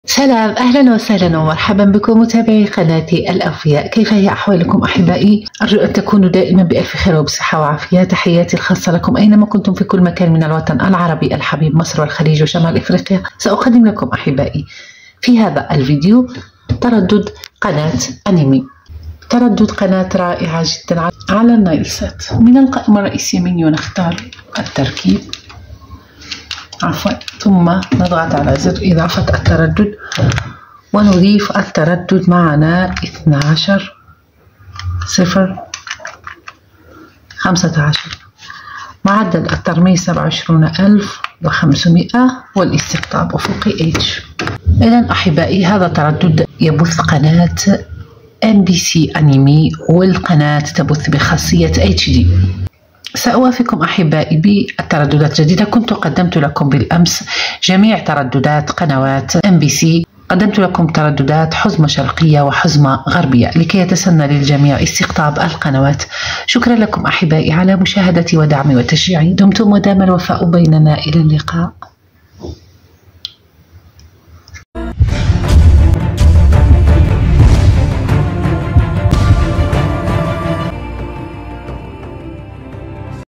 متعة تردد قناة mbc anime على النايل سات 2024 | ترددات جديدة قنوات جديدة متعة
استمتع بمشاهدة MBC Anime: دليل ردد قناة mbc anime وأفضل محتوى الأنمي  ; مشاهدة اقوي افلام ومسلسلات الانمي
في هذا الفيديو، سنأخذك في رحلة إلى عالم MBC Anime، وسنقدم لك دليلًا مفصلًا حول كيفية ضبط ردد قناة mbc anime على جهازك والاستمتاع بأفضل محتوى الأنمي المشوق افلام انمي One piece . سنقدم لك أيضًا نظرة عامة على البرامج والأعمال المميزة التي تقدمها القناة. انضم إلينا لاستكشاف عالم مثير من الأنمي عبر MBC Anime.
تردد MBC Anime و مشاهدة الأنمي في قناة MBC Anime